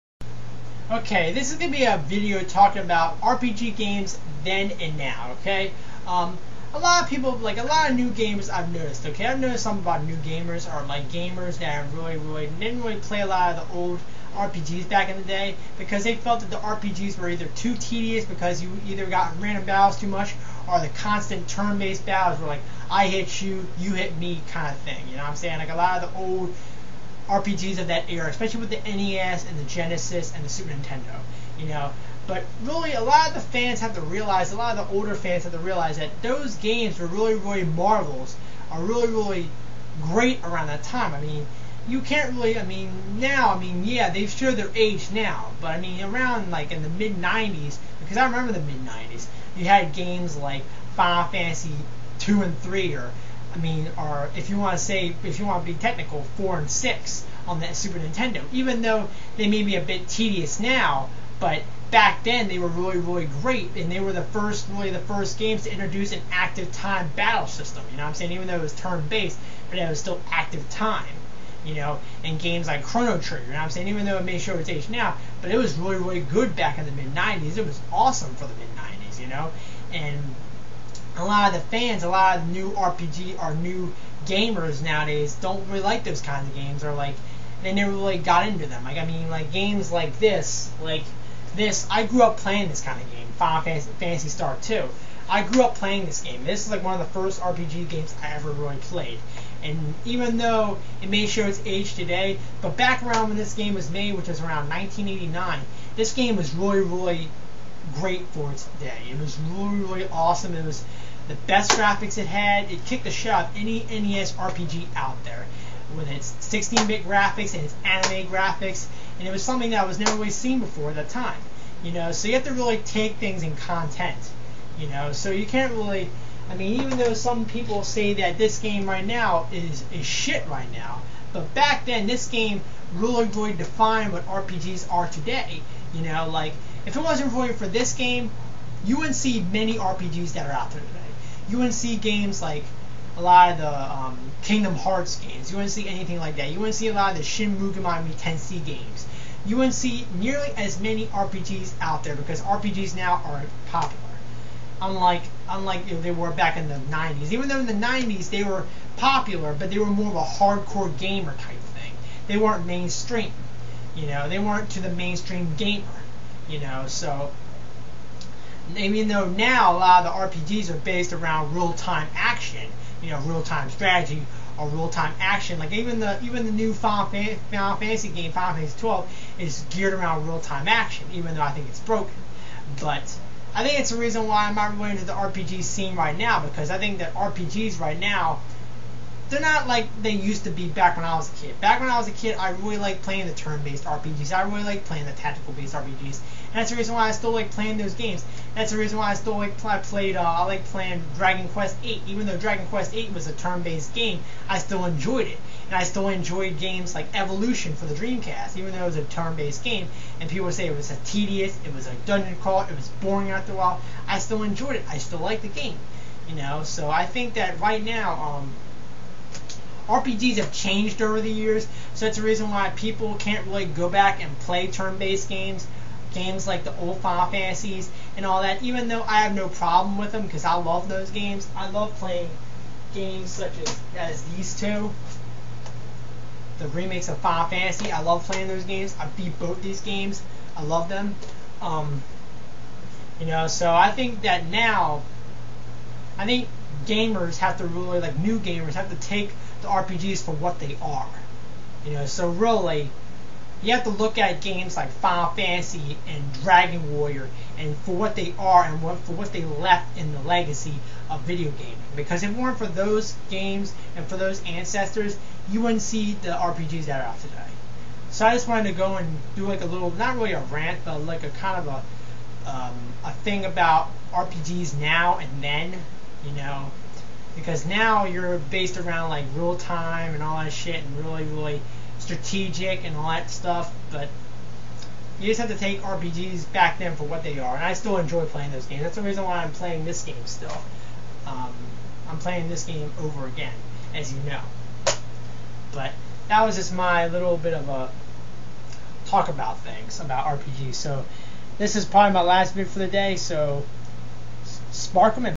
I talk about RPGs past and now..!!
August 14, 2007 | 56 likes, 7 dislikes
A brief message talking about RPGs of the past and the present ones.